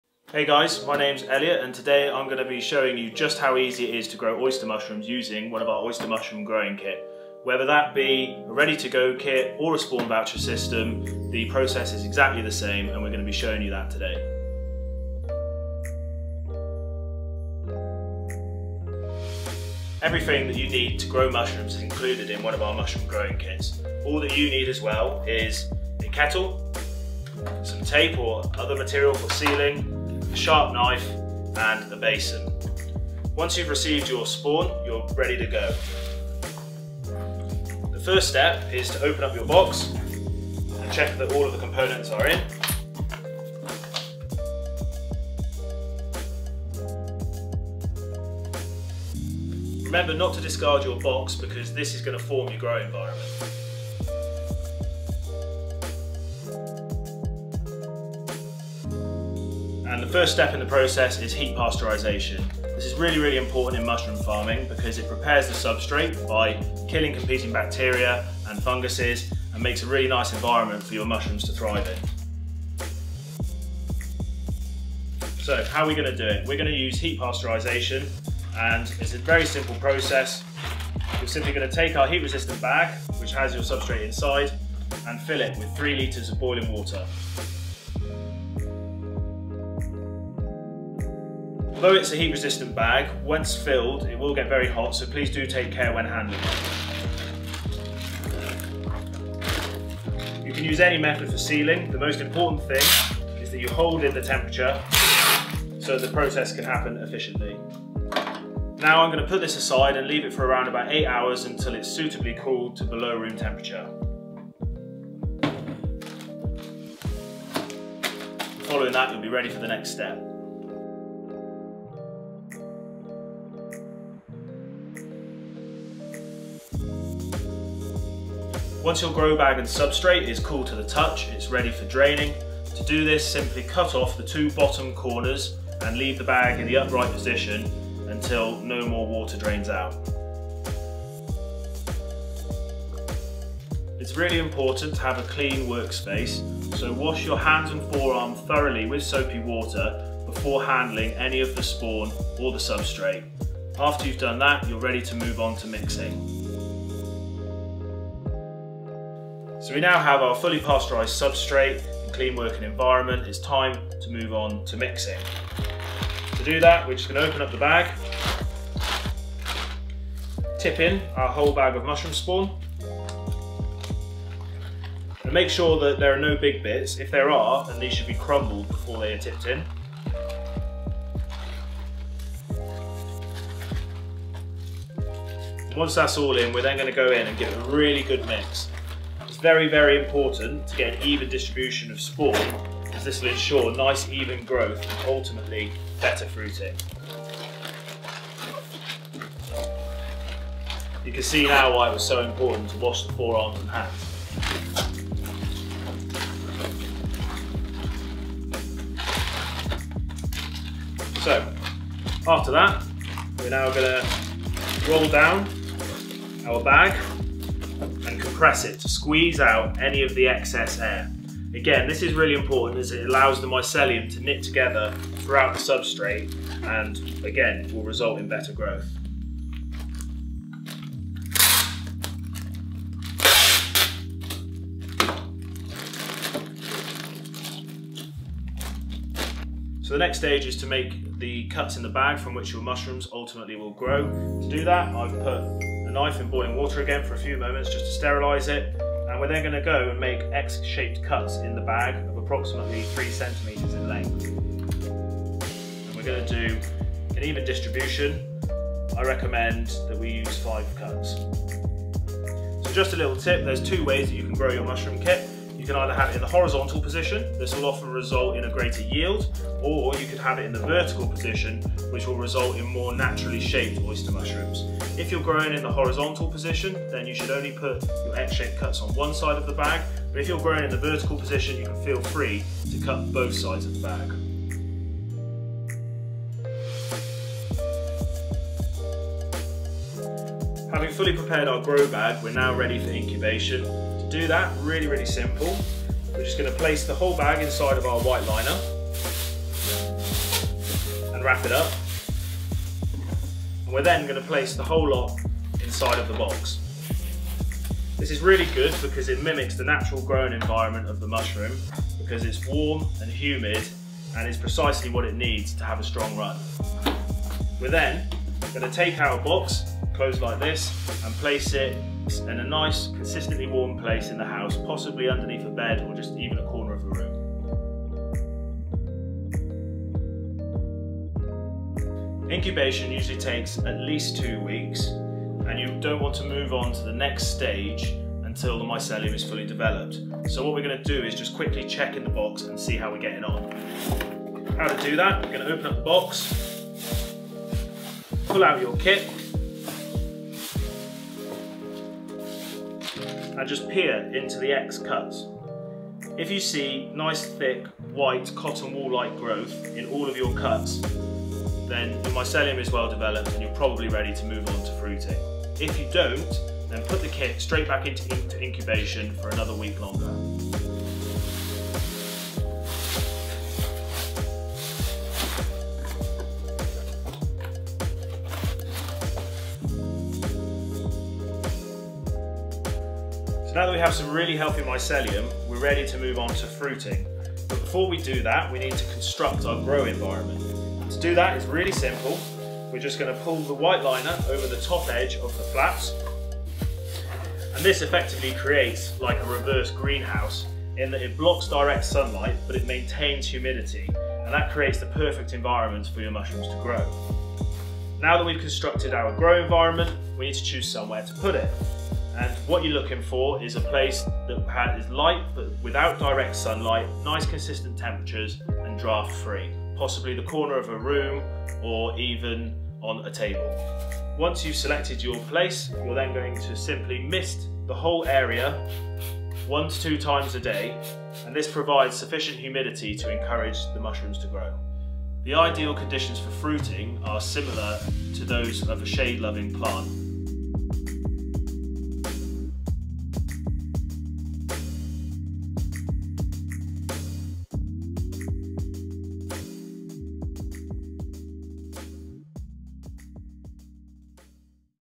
How To Grow Mushrooms From a Mushroom Growing Kit (Step By Step Guide) | Urban Farm-It's Classroom
NOTE: Some kits do not contain fresh spawn initially. If you have a gift or voucher kit, please claim your free spawn before beginning

Grow your own mushrooms anywhere! This video is a step by step guide on how to successfully set up and grow your own oyster mushrooms at home using Urban Farm-Its mushroom bag grow kit.

0:00 Introduction
0:18 Preparation
0:56 Pasteurisation
2:37 Mixing/Inoculation
4:45 Incubation
6:20 Fruiting